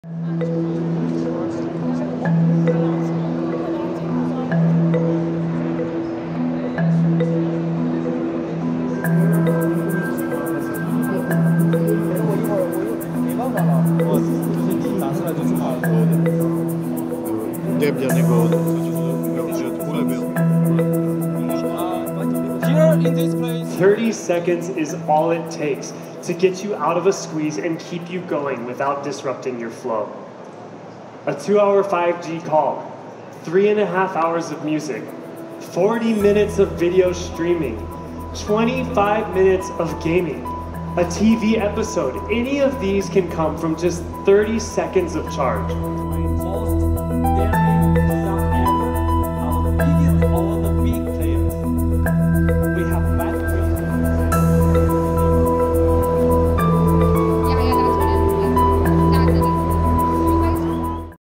This phone has a 9-minute charge time
Chinese smartphone maker Realme unveiled the GT3 smartphone which it says can be fully charged in just over nine minutes.